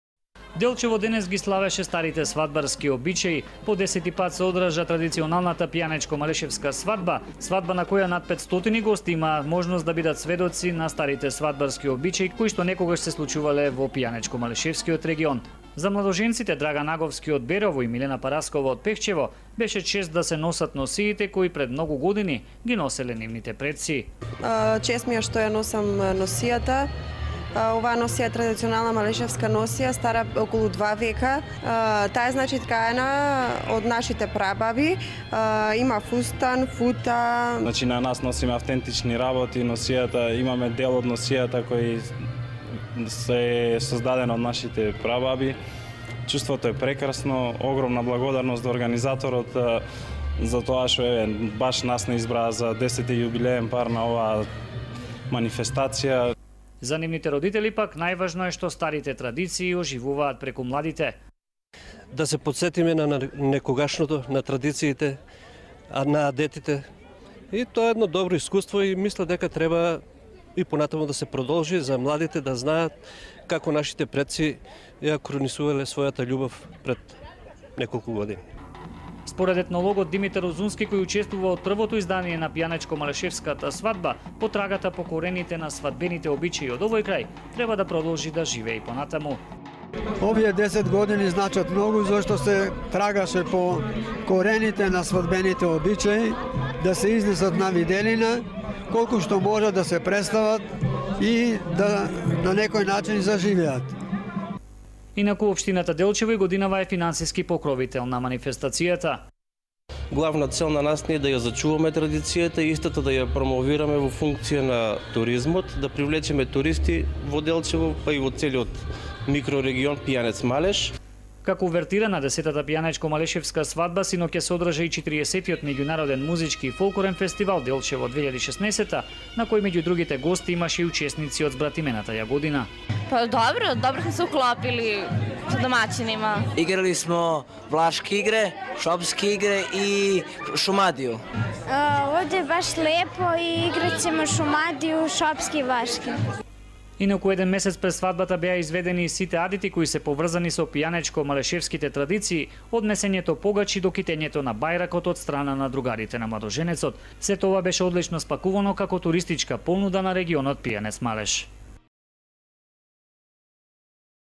Пијанечко-Малешевска свадба во Делчeво. 2016
Со стари свадбарски обичаи денеска во Делчево се одржа 10. јубилејна Пијанечко-малешевска свадба на која младоженци беа Милена Параскова од Пехчево и Драган Аговски од Берово.
Невестата и младоженецот носеа раритетни малешевски носии стари скоро два века, ткаени од нивните прабаби.
За Милена и Драган, денешниот ден претставува гордост и можност да ја продолжат старата традиција. – Носиме стари носии подготвени од нашите прабаби. Ова се стари материјали кои и нашите родители, а и ние ги чувавме и горди сме што денеска со нас продолжува оваа убава традиција на стара макеоднска свадба, изјавија Милена и Драган.
Беа изведени дел од обичаите, како китење на бајракот, подготвување на младоженецот, а беше изиграно и големото свадбарско оро водено од невестата.
Според фолклористот Димитар Узински, оваа традиција со свадбата е од големо значење за сочувување на свадбарските обичаи од регионот.
– Свадбата има големо значење, бидејќи денеска многу малку од обичаите се почитуваат на модерните свадби. Во овие 10 години трагавме по заборавените обичаи и горд сум што успеавме да сочуваме дел, но наша задача е и понатаму да продолжиме со потрагата по обичаите, бидејќи тие го носат нашиот индентитет. Убава е оваа прилика за младите да видат како се правела некогаш свадбата со сите овие обичаи, рече Узунски, кој 10 години соработува со КУД „Гоце Делчев“ кој е организатор на Пијанечко-малешевската свадба.
Младите имаа прием и во Општина Делчево. Советникот во општината Тони Стоименовски оцени дека свадбата во овие 10 години е изградена како бренд на регионот.
– Како општина даваме максимум за да ја поддржиме во секоја смисла оваа убава традиција која ја пакуваме како туристичка понуда од регионот, за да привлечеме гости, но и да дадеме свој придонес во сочувањето на старата традиција, рече Стоименовски.
Кореографот од КУД Гоце Делчев, Зенил Реџепов е задоволен што како организатори успеале да ја одржат оваа традиција и да ги сочуваат старите свадбарски обичаи. – Овие 10 години максимално работиме на тоа поле, а најавувам дека ќе продолжиме и понатаму, рече Реџепов.
Манифестацијата почна синоќа со почетокот на 40. Фолклорен фестивал на кој учествуваа 15 фолклорни групи од земјава и регионот.
Традиционалната свадба и фестивалот се поддржани од Министерството за култура и од Општина Делчево.